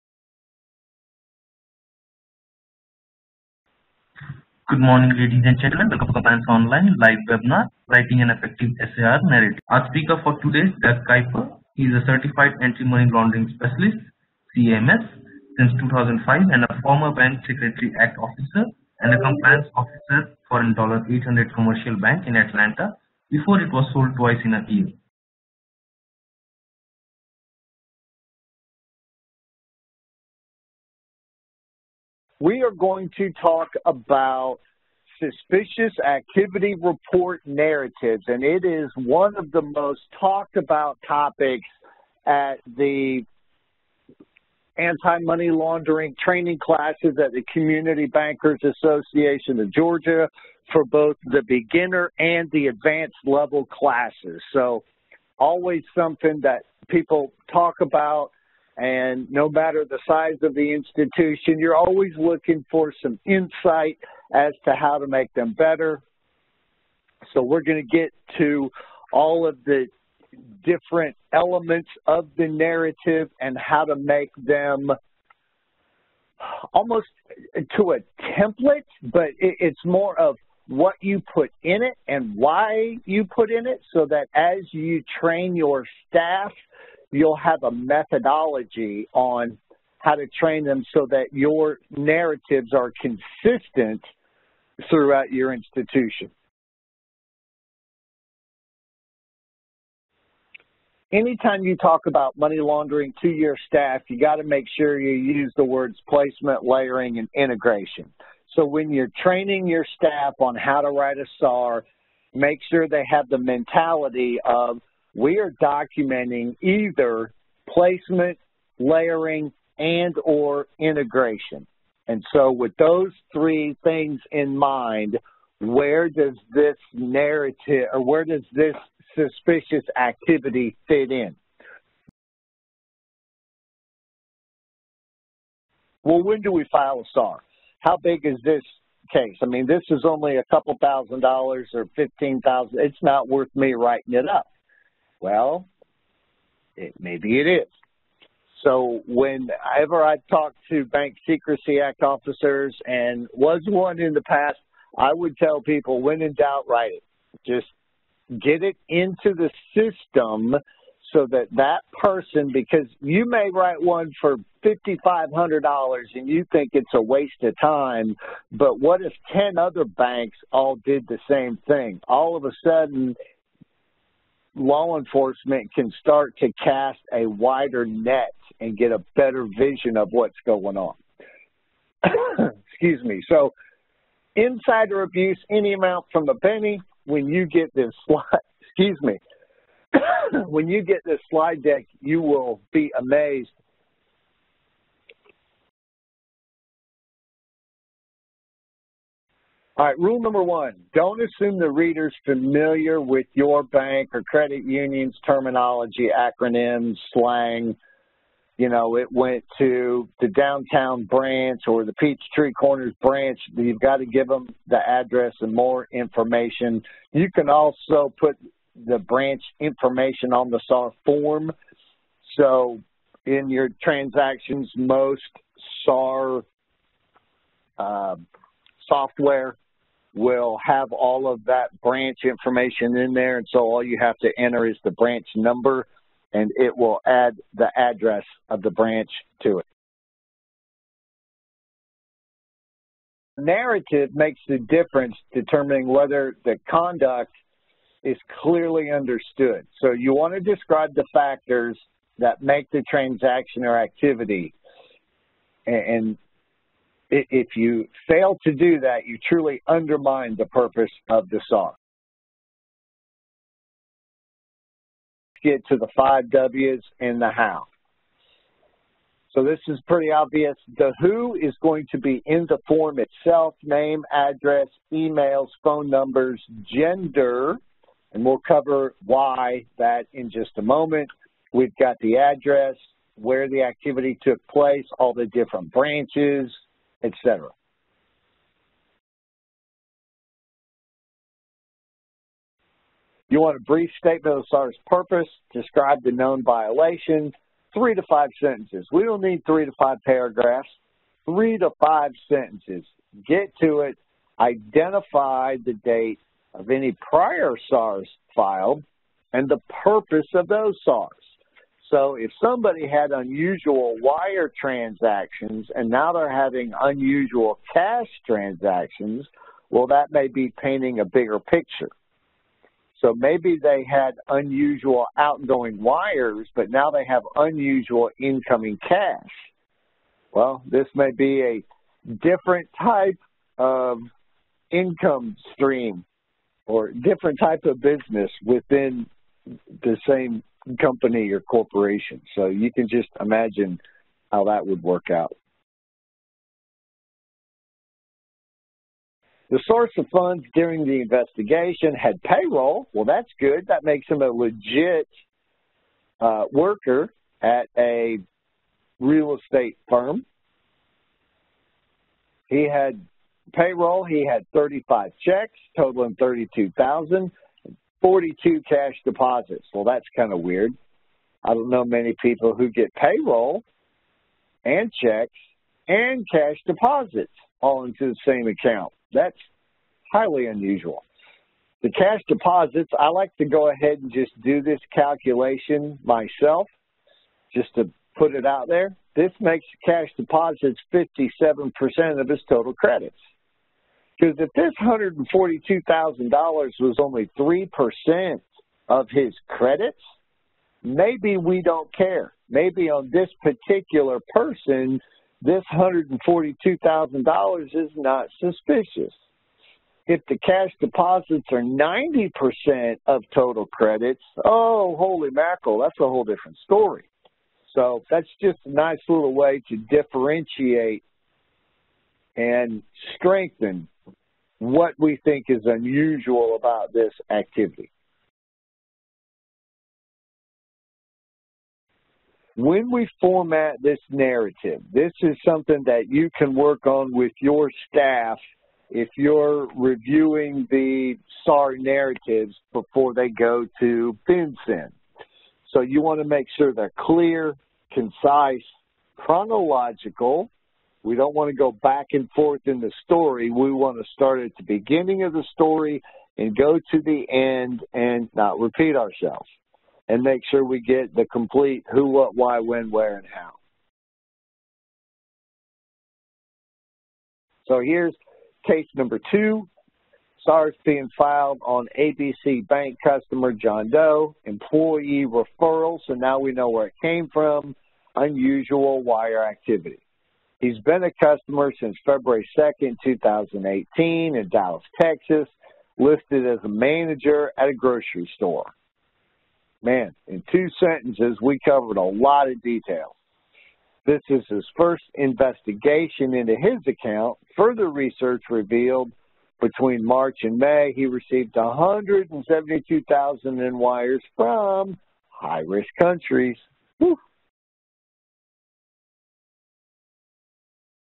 Webinar on Writing an Effective SAR Narrative
This training on Suspicious Activity Reporting (SAR) will provide details on how to prepare a SAR (Suspicious Activity Reporting) narrative that meets examiner scrutiny and provides necessary detail for law enforcement.

You will learn:
The 4 C’s: Clear, Concise, Chronological and Complete
The 5 W’s: Who, What, When, Where and Why
How did the activity occur?
Tell the complete story by fitting the pieces together